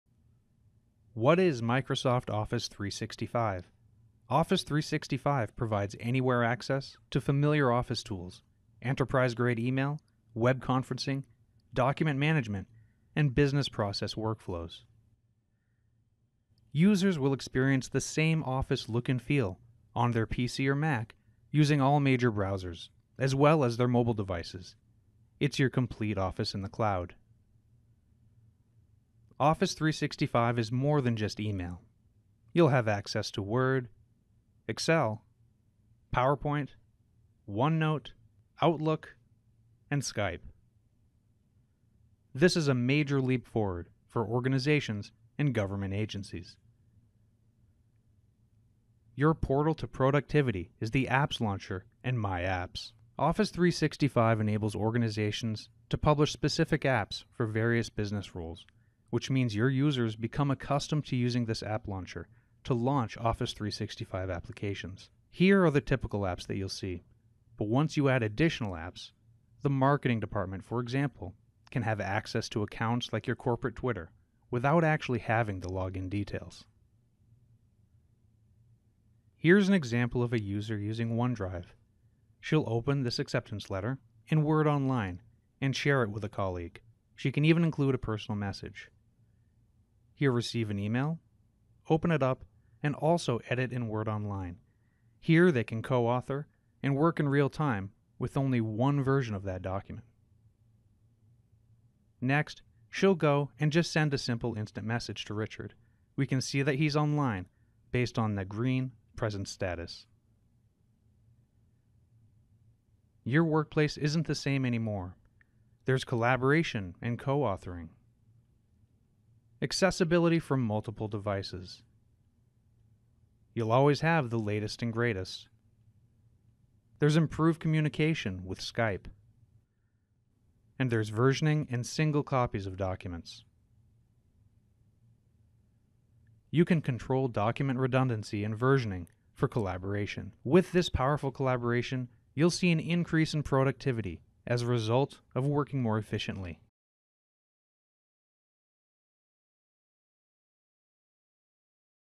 What Can You Do with Office 365
Microsoft Office 365 is your Office in the cloud! Use a dynamic set of cloud connected tools to help you communicate, collaborate, be more productive and efficient. You can depend on familiar apps like Word, Excel, PowerPoint, Outlook and Skype. Check out this video to learn all about what Office 365 has to offer!